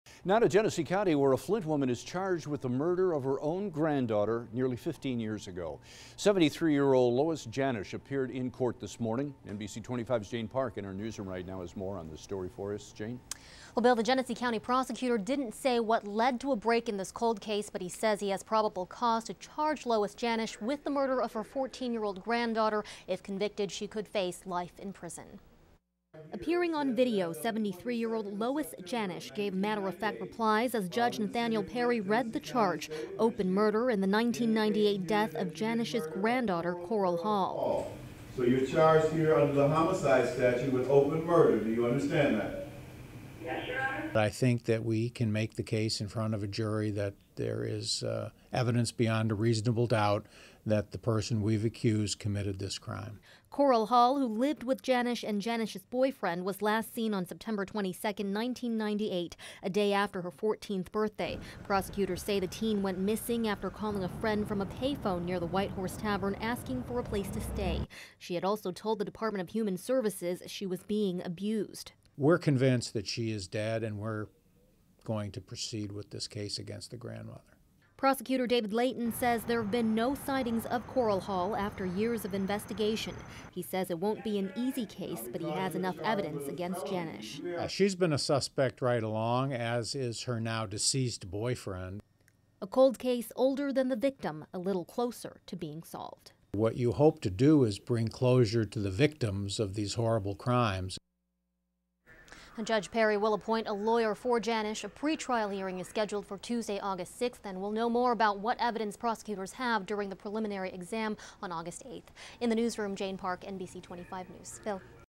Flint grandmother charged with granddaughter's murder
NBC25's Jane Park reports.